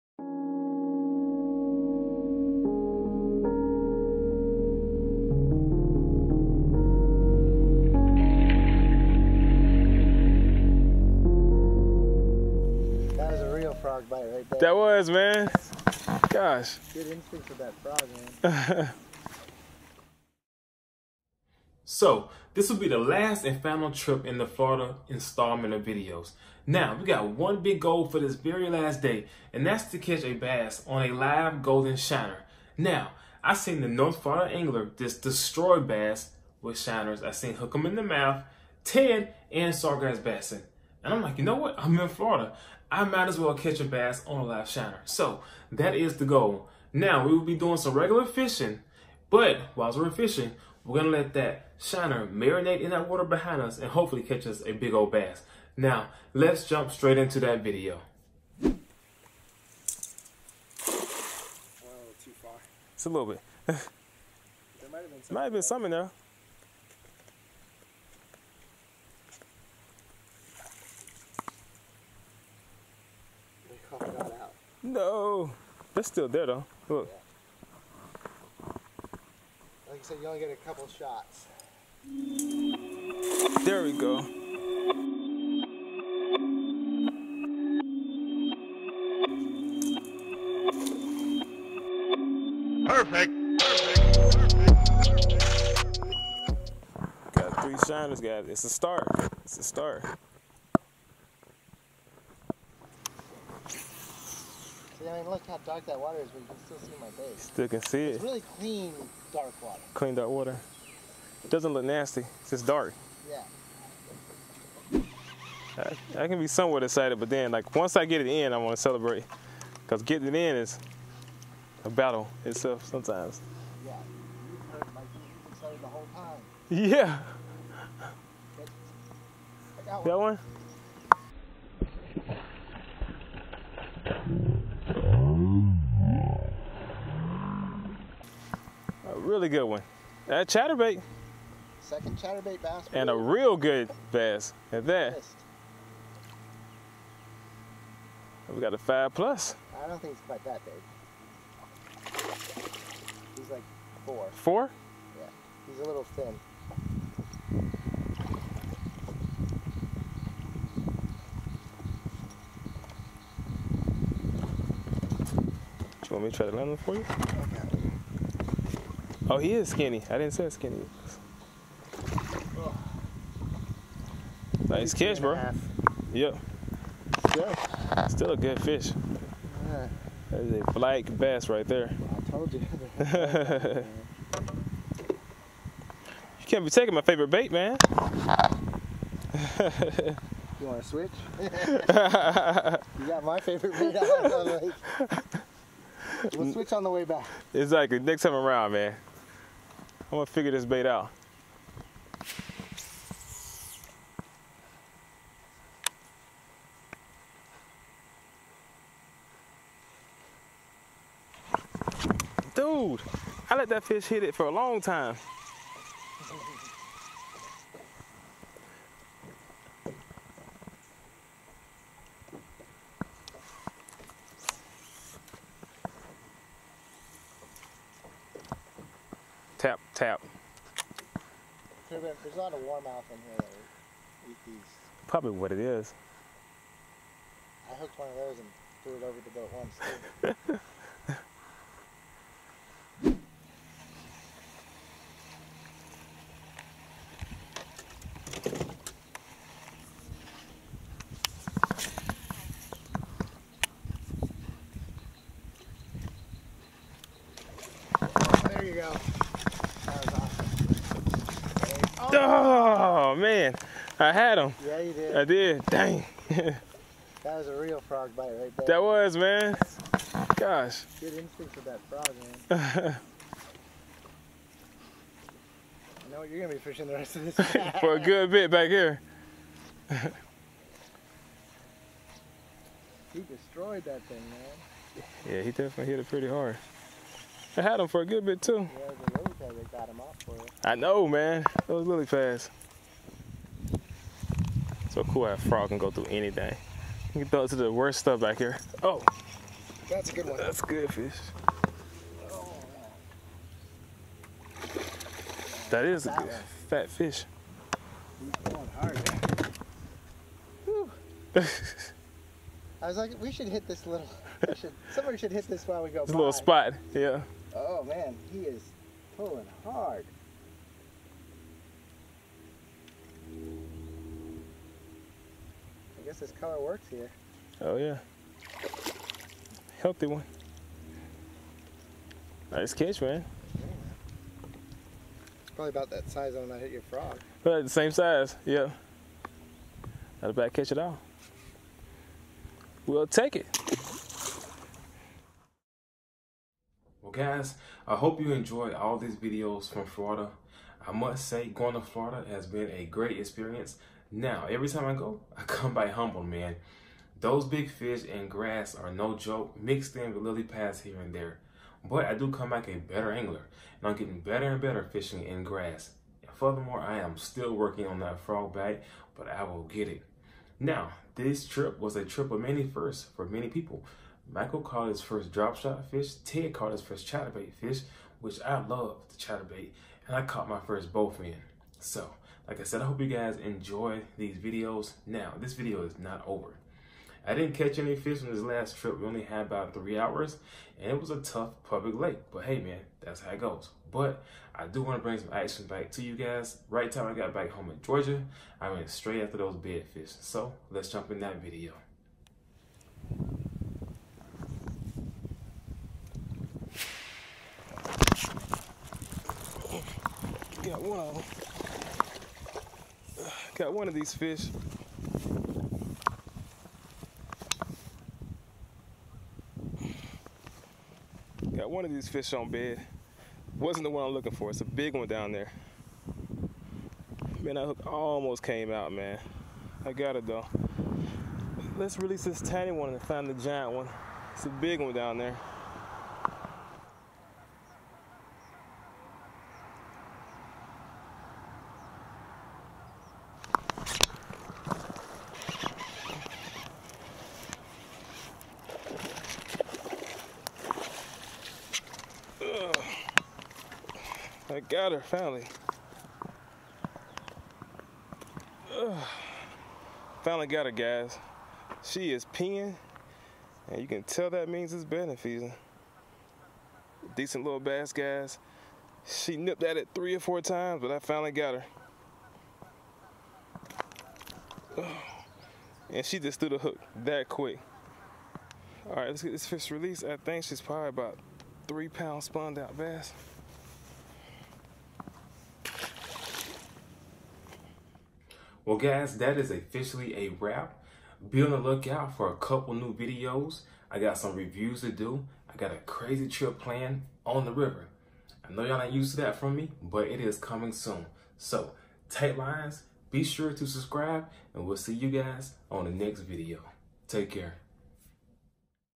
Fishing Super BLACK WATER for Bass!!
This was one cool lake to fish!! I have never fished a lake with water so black. We didn't have long to fish, but we made the best of it. Thanks for all the support guys, I hope you enjoyed the Florida Series!!

SOCIAL MEDIA SPOTS:

FISH BRAIN:  Follow Your Boy!!